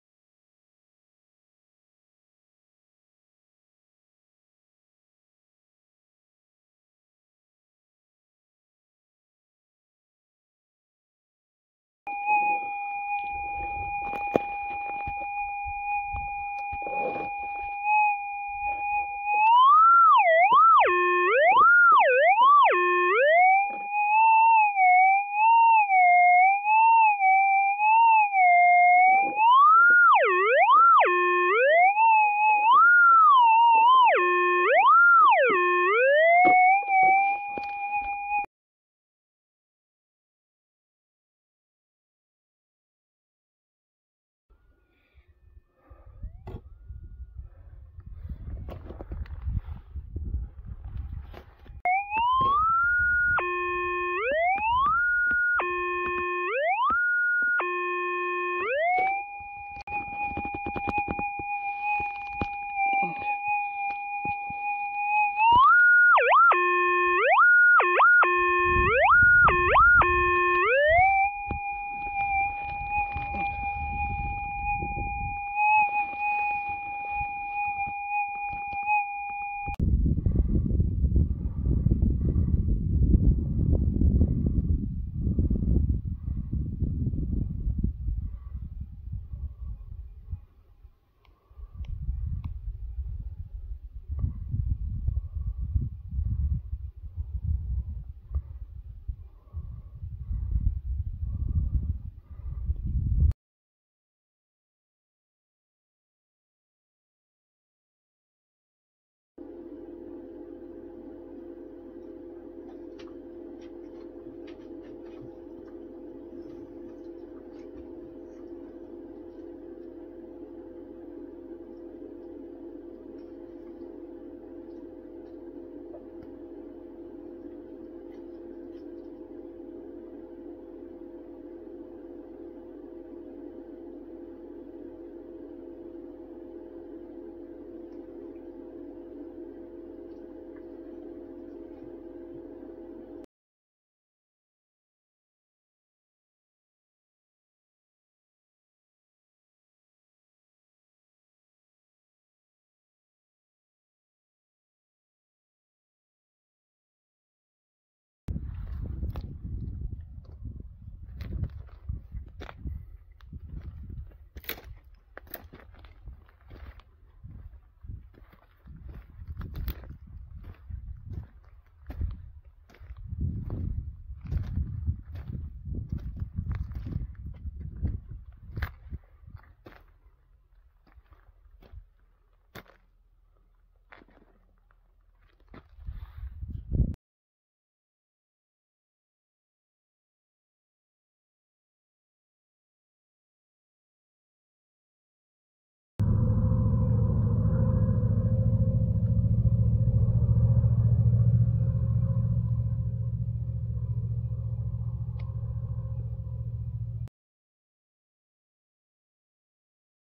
I found old Yella (Gold) Laverton July 2023, GPX6000 & GPZ7000
Hunting old yella off Bandya Rd Laverton.
Working a pending Exploration Lease, I discover little locations of yella.